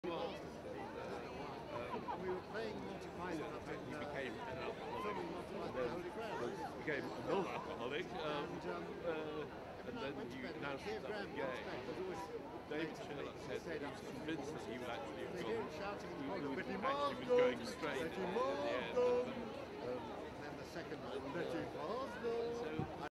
Terry Jones at 'A Liar's Autobiography' Premiere: 56th BF...
Terry Jones at 'A Liar's Autobiography' Premiere: 56th BFI London Film Festival at Empire Leicester Square on October 16, 2012 in London, England.